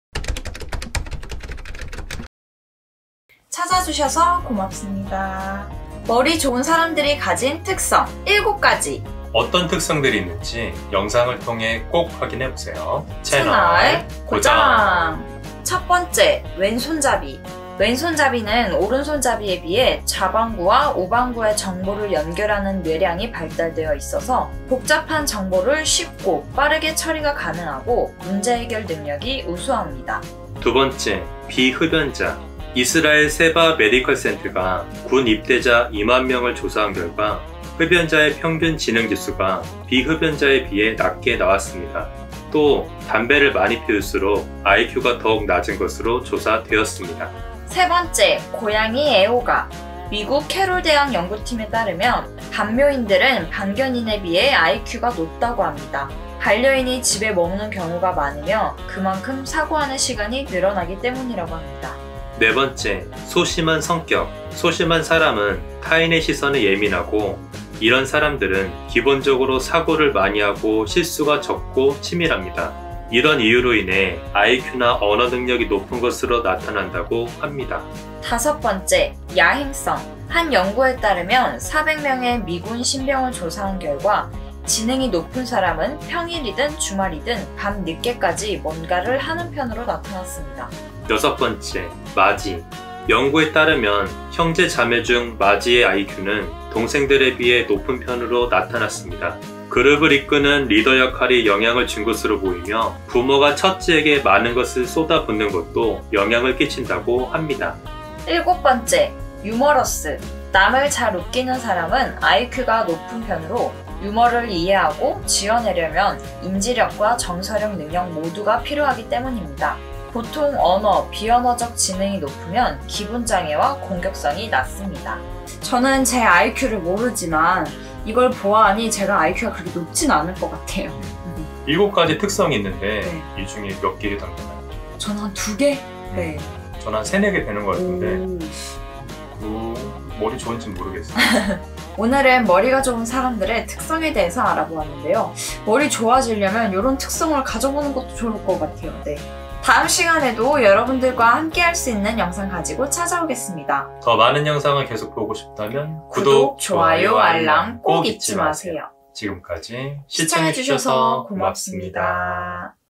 머리 좋은 사람들이 가진 특성 7
머리좋은사람들이 가지는 특성이 있다는데 ... 나는 몇개나 해당될까?!

체크해보세요!